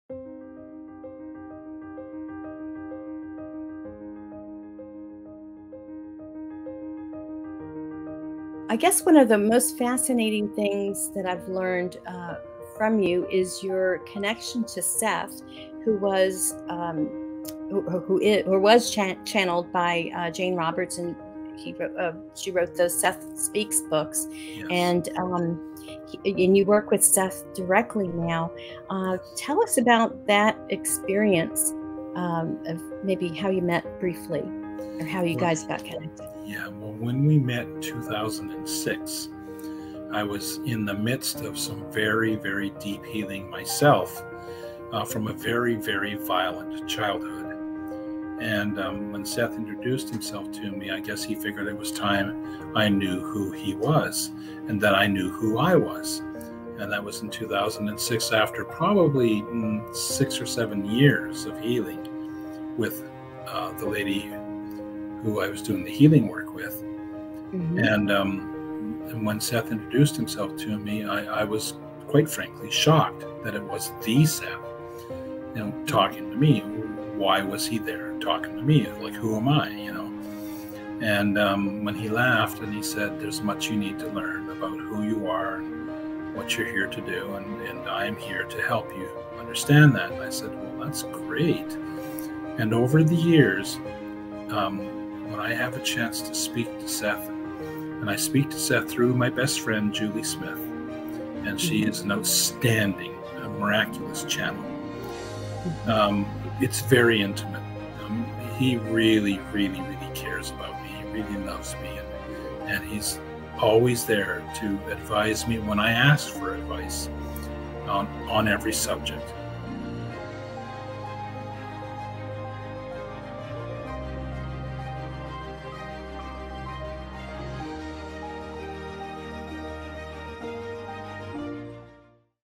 How the Channelled Spiritual Teacher Seth Opened This Healer's Eyes to His Gift
Coming up on Good-Vibes.TV... Quantum Emotional Healing and Seth!